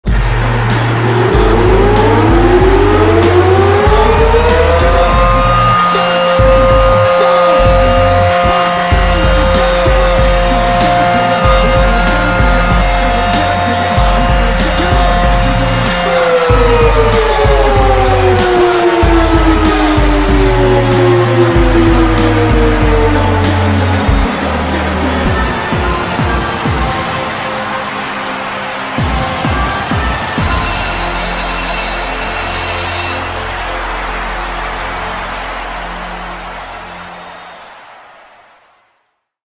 New York Yankees 2022 Home Run Siren
New York Yankees home run siren used during the 2022 MLB season at Yankee Stadium in The Bronx, NY.